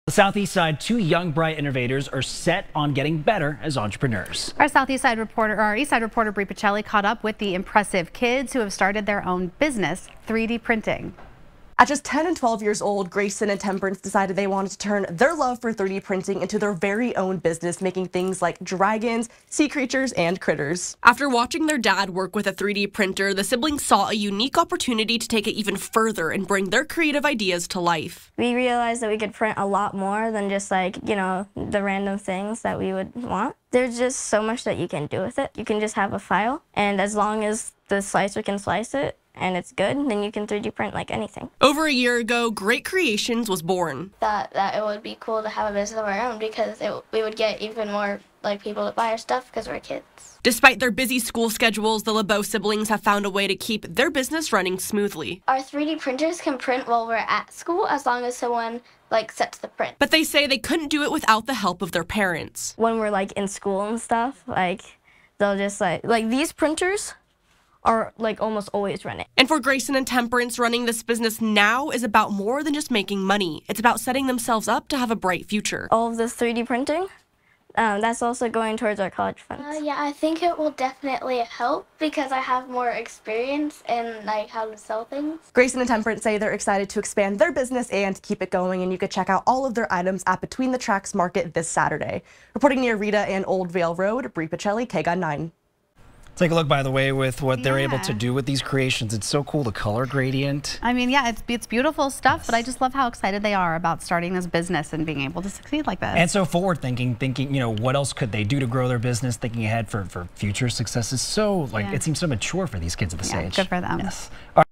Local children launch 3D printing business
Grayson Laboa, 12, and Temperance Laboa, 10, have created their own business making 3D-printed items like dragons, critters, and sea creatures.